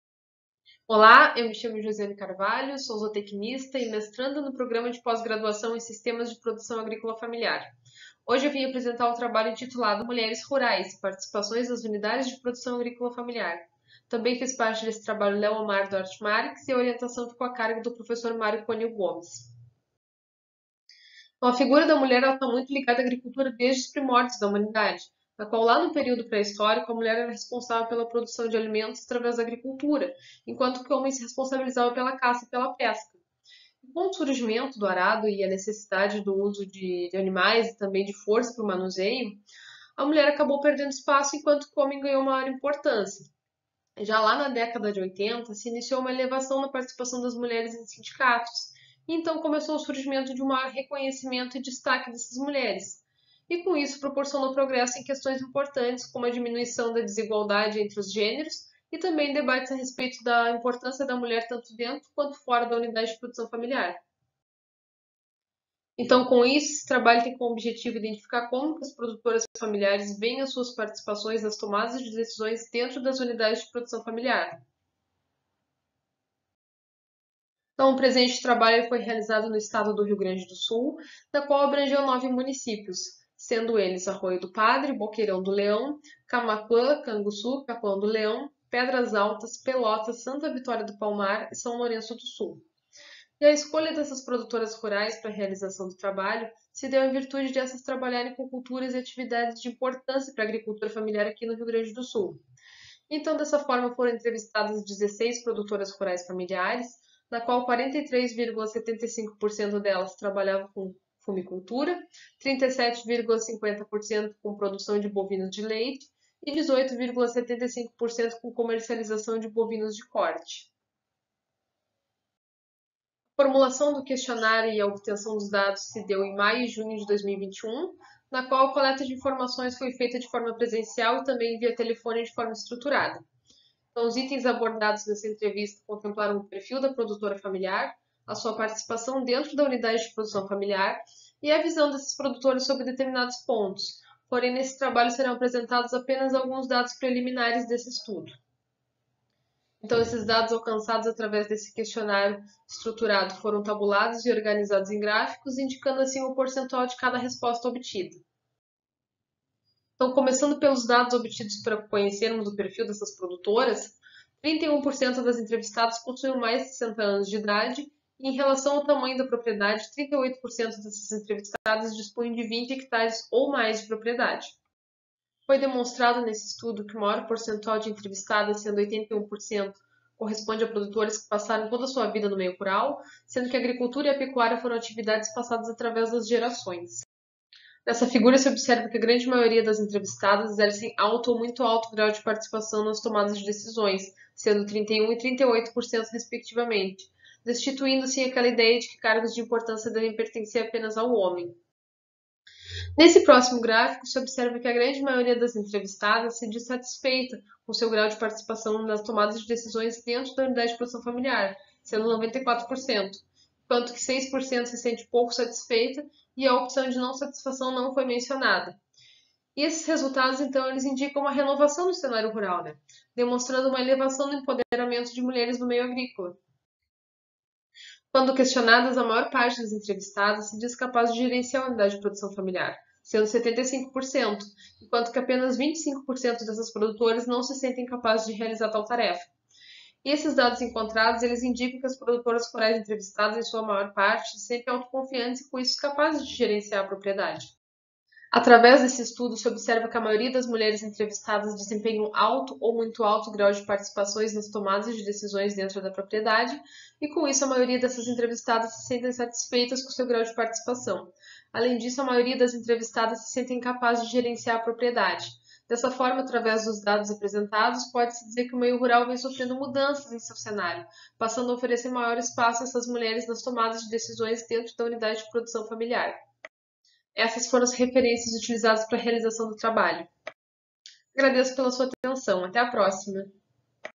“VII SIIEPE - UFPEL - JOSIANE DUARTE DE CARVALHO"
7ª Semana Integrada UFPEL - XXIII Encontro de Pós-Graduação
Área: Ciências agrárias
Título do trabalho: Mulheres rurais: Participações nas unidades de produção agrícola familiar
Descrição: O trabalho tem como objetivo identificar como as produtoras familiares veem suas participações nas tomadas de decisões dentro das unidades de produção agrícola familiar.